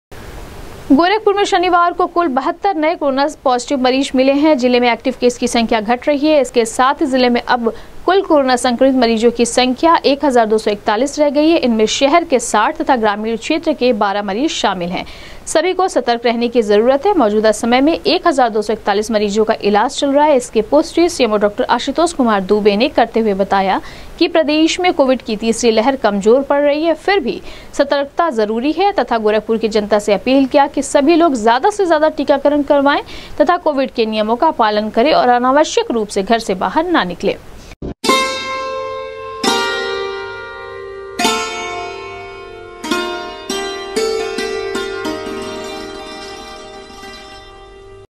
गोरखपुर में सोमवार को पाए गए 72 नए कोरोना संक्रमित मरीज, जिले में घट रही है एक्टिव केस की संख्या, अब
गोरखपुर में सोमवार को पाए गए 72 नए कोरोना संक्रमित मरीज, जिले में घट रही है एक्टिव केस की संख्या, अब एक्टिव केस की संख्या हुई 1,241,सीएमओ डॉक्टर आशुतोष कुमार दुबे ने की पुष्टि